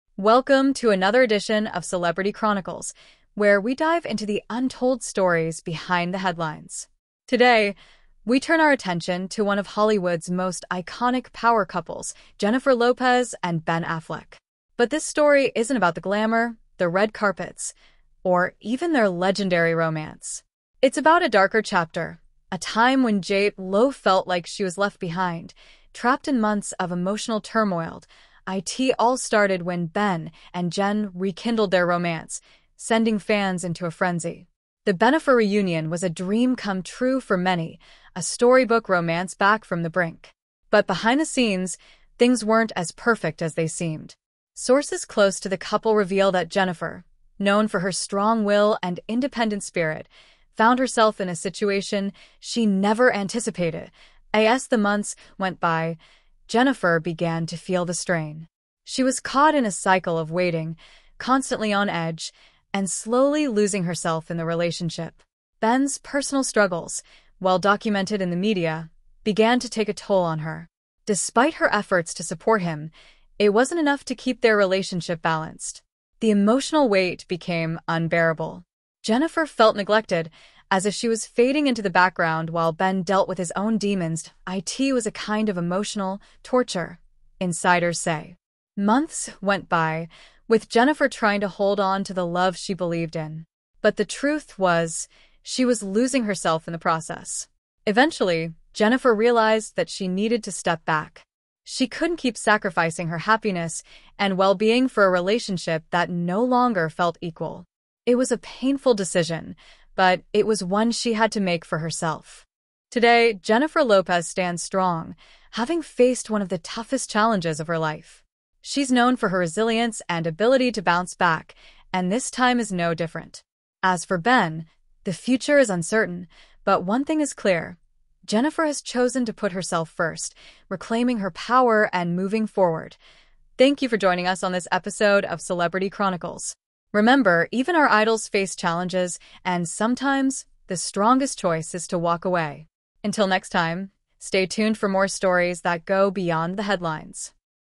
Jennifer Lopez Was ‘Left Behind’ by Ben Affleck After ‘Torture’!
Jennifer Lopez was ‘left behind’ by Ben Affleck after months of ‘torture.’ The couple, who were once the epitome of Hollywood romance, have faced severe turmoil, leading to an emotional rollercoaster for J.Lo. As more details emerge, fans are left shocked at the behind-the-scenes drama that unfolded between these two icons. This video dives deep into the heartbreaking journey of Jennifer Lopez and Ben Affleck, revealing the truth about their troubled relationship and the emotional ‘torture’ that left J.Lo ‘left behind.’ Watch as we uncover exclusive details and shocking revelations about what really happened between the superstar couple. Will Jennifer Lopez ever recover from this? Can their relationship survive after such a harrowing experience?